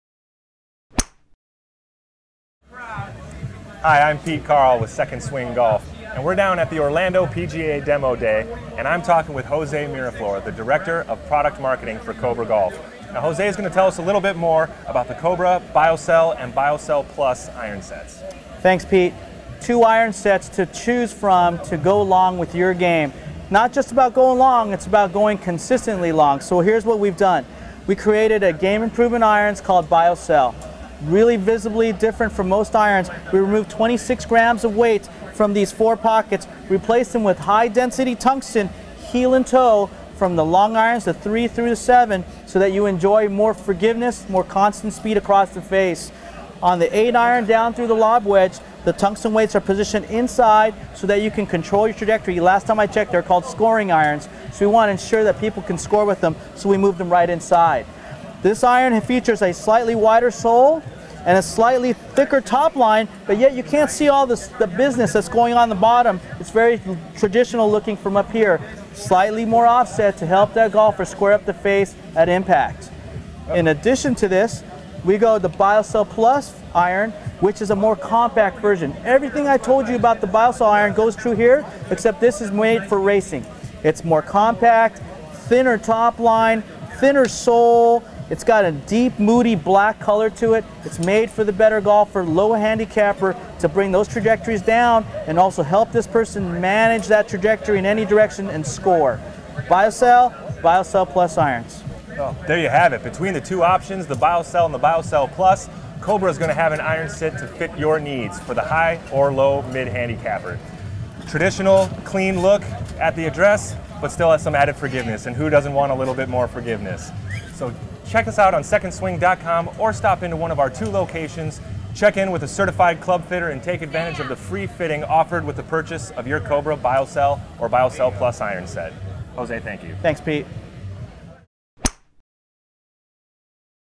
COBRA BiO CELL Irons Review and COBRA BiO CELL+ Irons Review
2nd Swing Golf club expert Pete Karle reviews the 2014 COBRA BiO CELL Irons and 2014 COBRA BiO CELL+ Irons with Jose Miraflor, COBRA Golf director of product marketing.